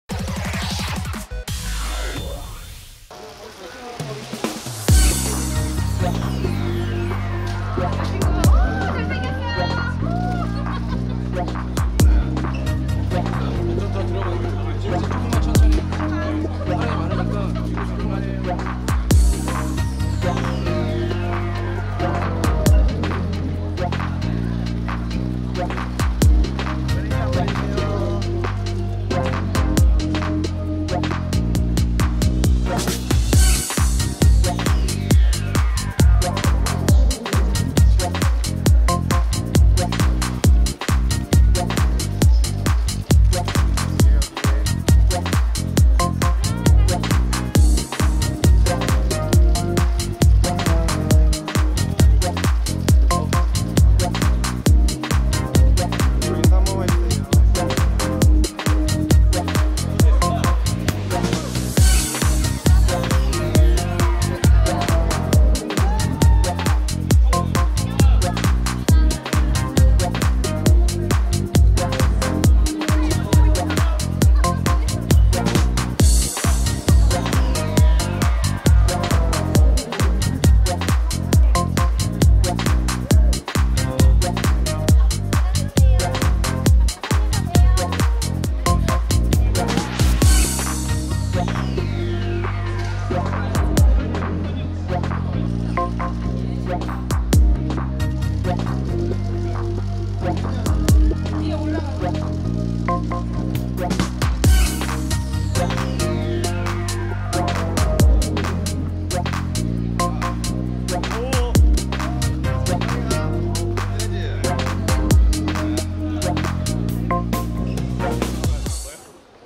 여진구·최진혁·이지훈, 빛이 나는 외모 (Yeo jin goo·Choi jin hyuk·Lee ji hoon, GMP INT' Airport Departure)
17일 오후 배우 여진구, 최진혁, 이지훈이 KCON 2019 JAPAN 참석차 김포공항을 통해 일본으로 출국했다.